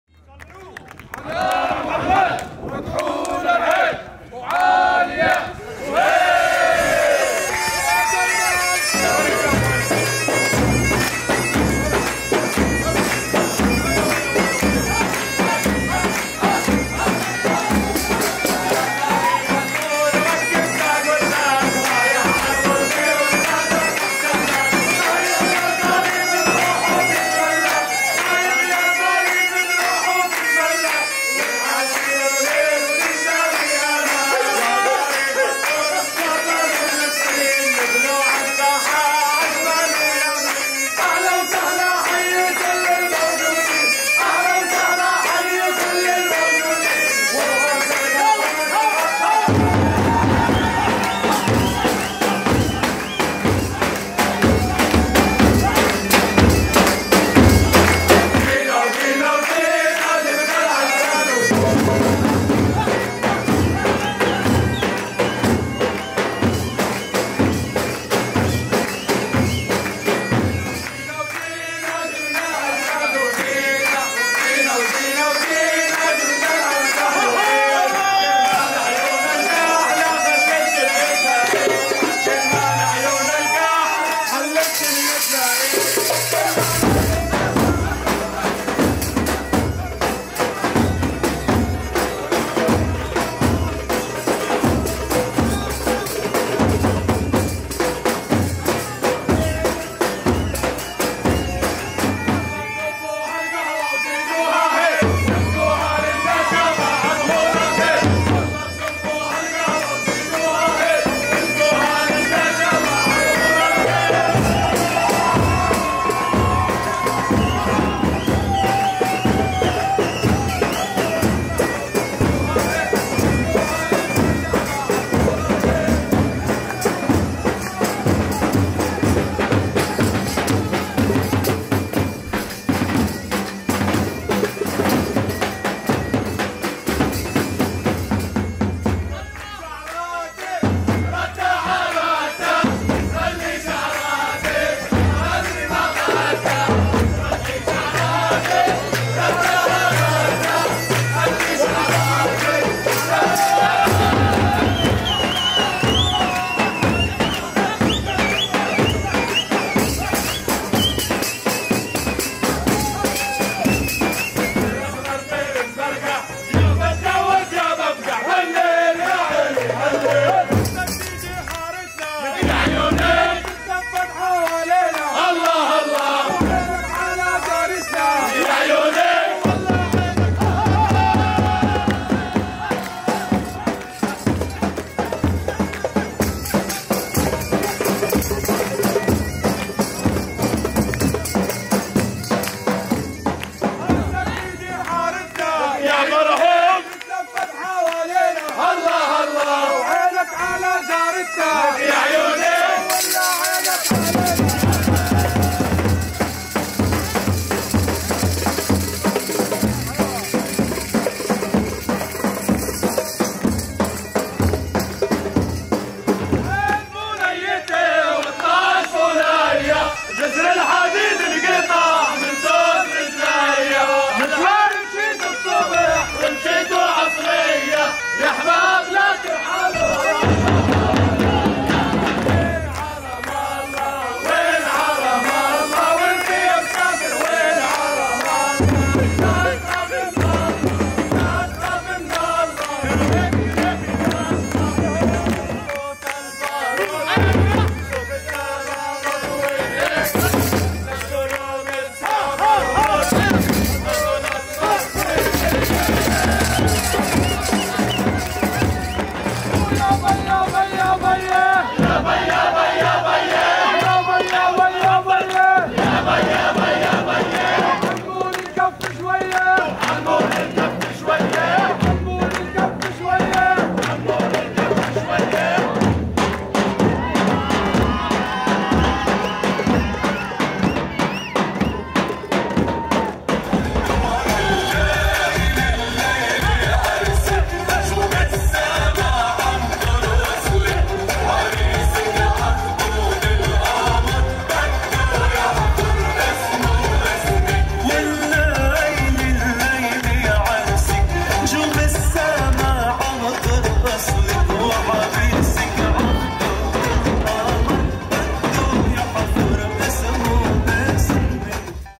Fusion Zaffa - Palestinian wedding performance.
Fusion Zaffa - Performing live grand entrance at a Palestinian wedding including bagpipe, drummers, chant singer, and dabke dancers.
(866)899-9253
follow us @fusionzaffa